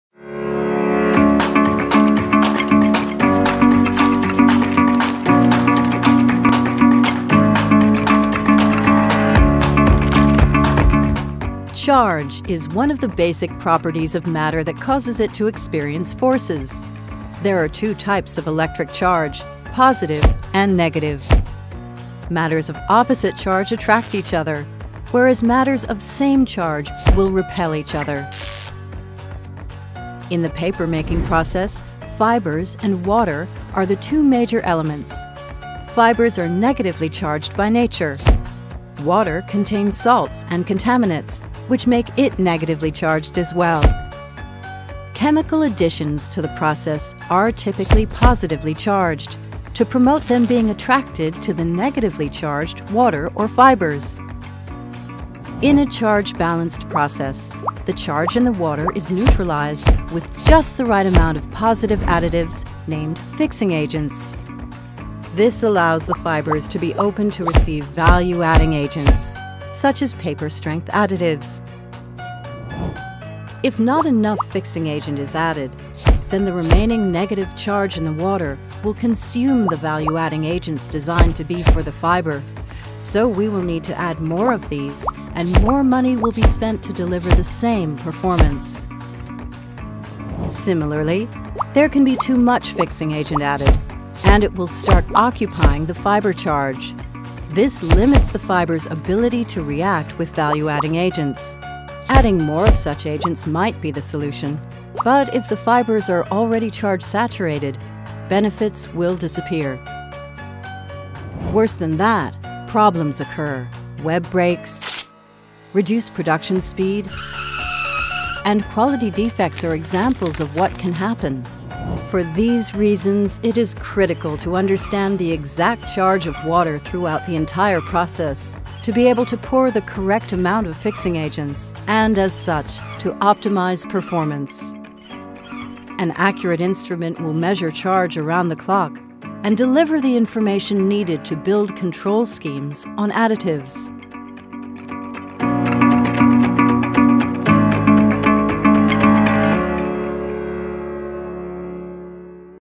BTG Learning Academy - CHARGE
Discover what is Charge in the paper-making process.